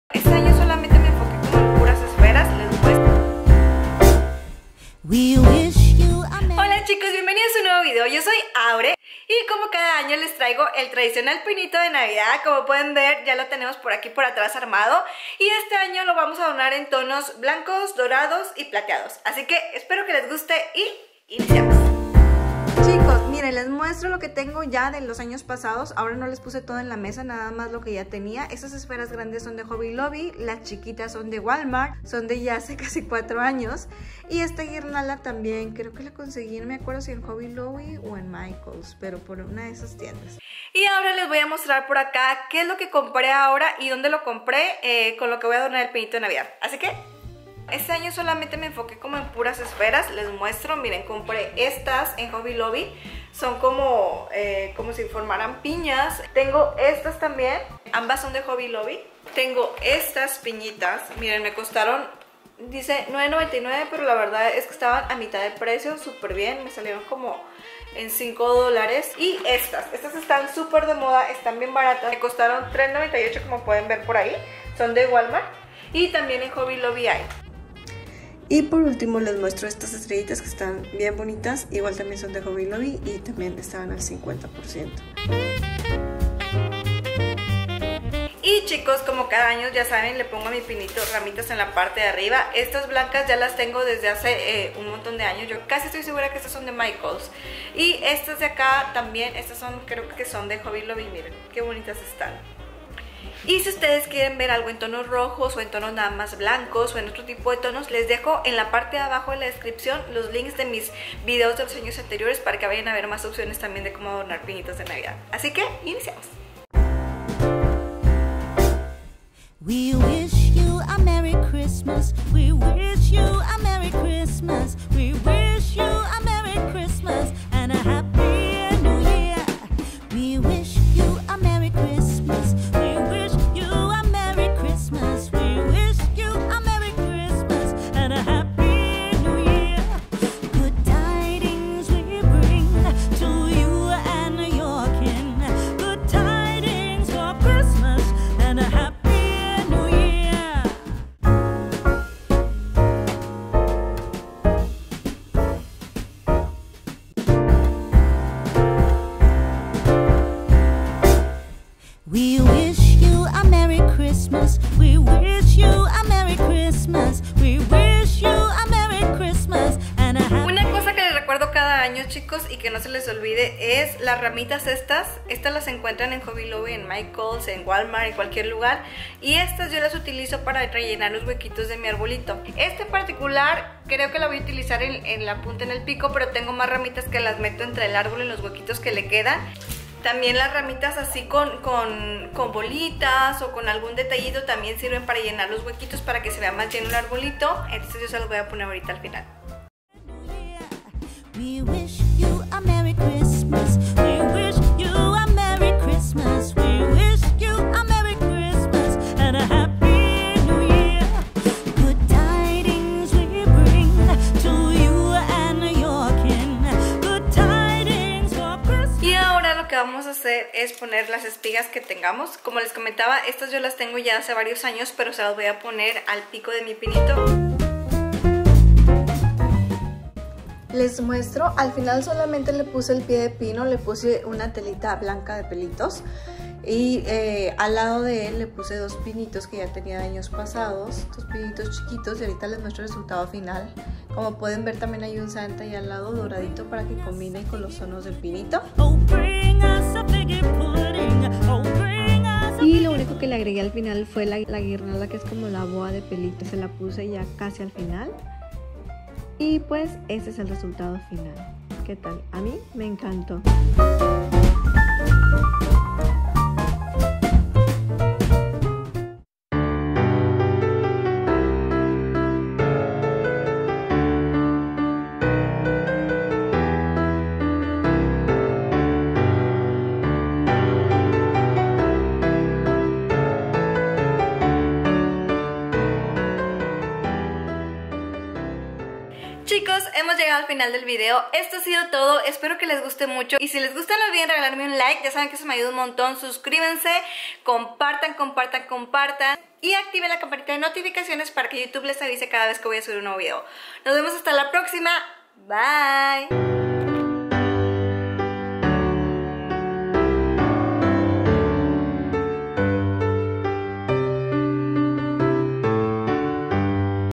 COMO DECORAR UN ARBOL DE NAVIDAD BLANCO CON DORADO 🎄🎅
Los invito a ver la decoración de mi arbolito blanco,dorado y plateado, espero que les guste y les de alguna idea para sus decoraciones